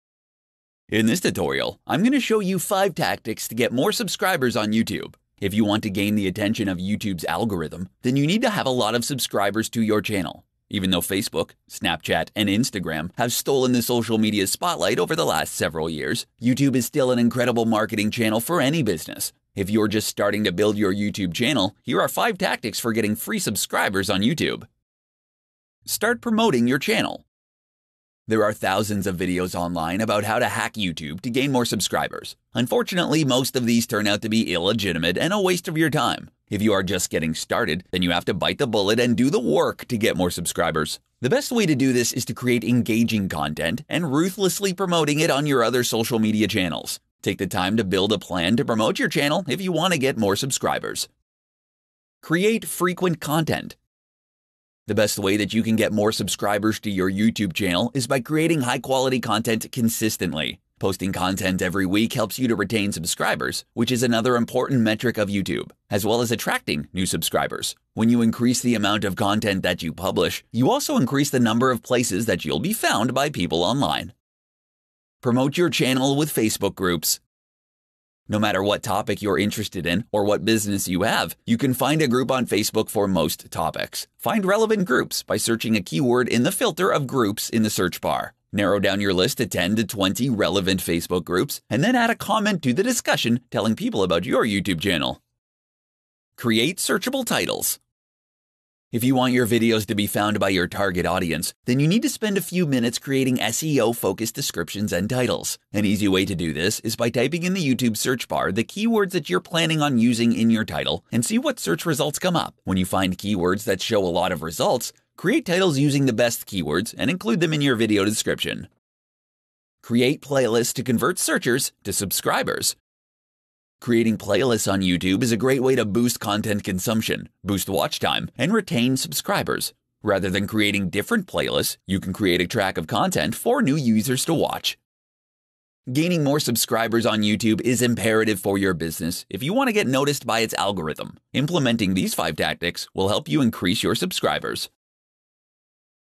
5 Ideas to Get More Subscribers on YouTube fast l Sporty l Learn
In this video we discuss about Five simple ideas to get more Subscribers on YouTube fast